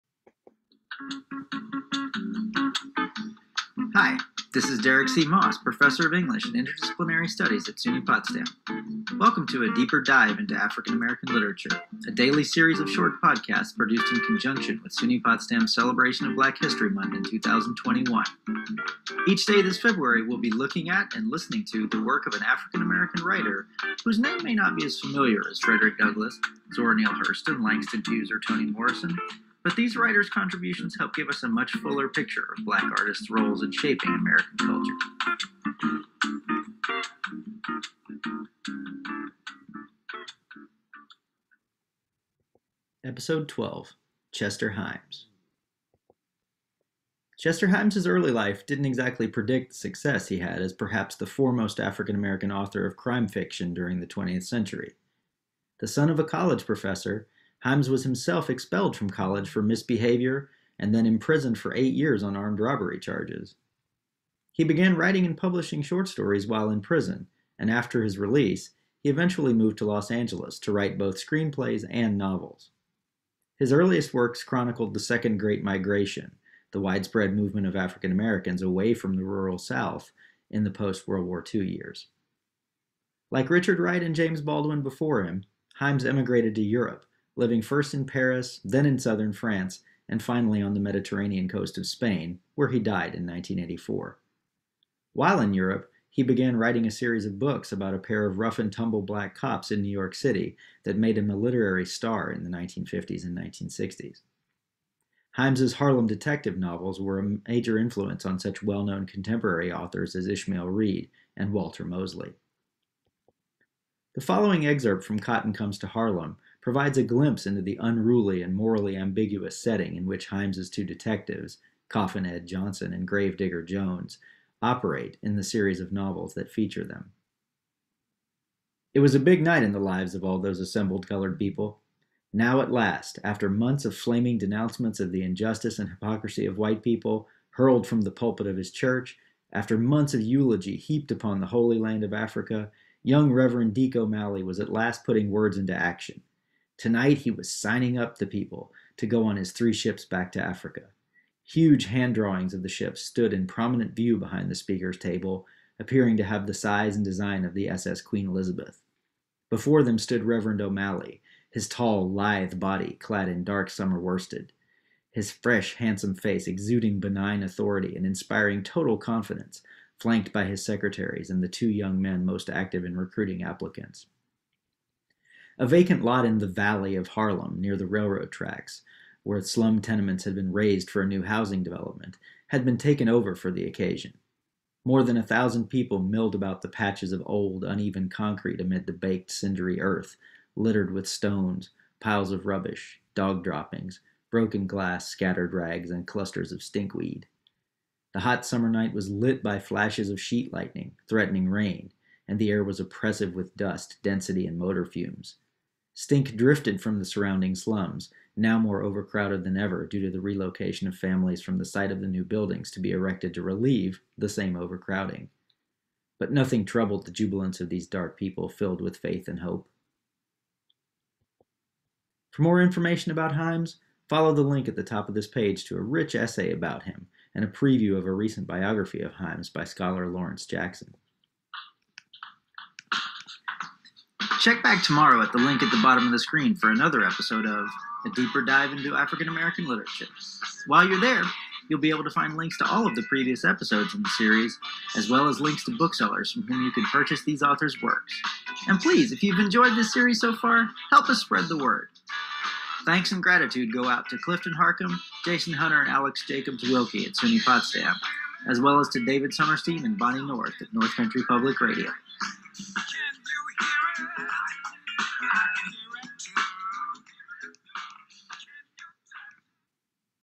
Chester Himes Black History Month Podcast
Some biographical details and a sample of the work of Chester Himes, one of the foremost African American authors of crime and detective fiction.

This podcast is part of the "A Deeper Dive into African American Literature" series that was produced in 2021 in conjunction with SUNY Potsdam's Black History Month celebration. For the other videos in the series, go

[NOTE: Excerpts from the author's work is used in accordance with Fair Use guidelines for academic purposes and there is no financial gain of any kind accruing from their use to either the producers of these videos or to SUNY Potsdam.]